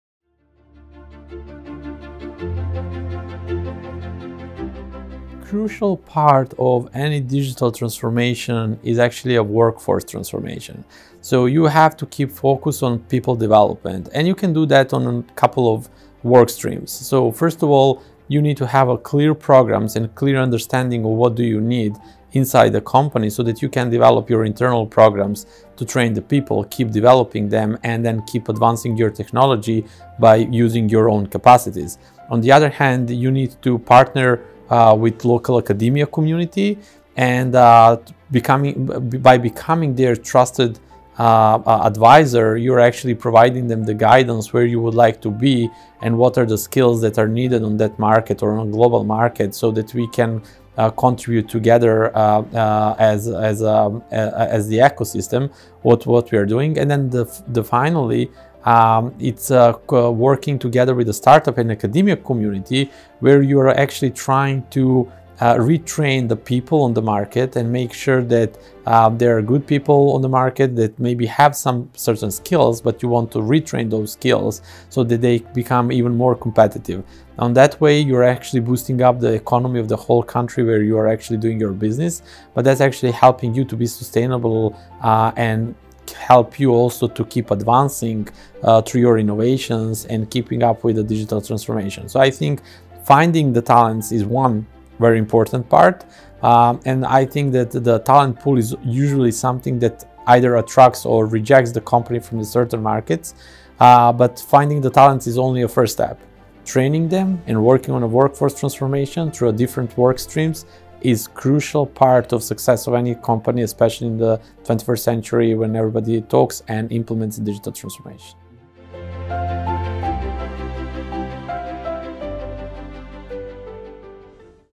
Workforce Transformation is Essential Key to Digital Transformation | NCR
Stefan Lazarevic, General Manager, NCR Serbia & EMEA External Affair shares his views on needed workforce transformation for digital transformation. Stefan made these remarks during the Digital Supply Chain Institute's 7th Executive Leadership Forum in Belgrade, Serbia. Around 70 supply chain executives attended the DSCI's seventh forum in September 2019.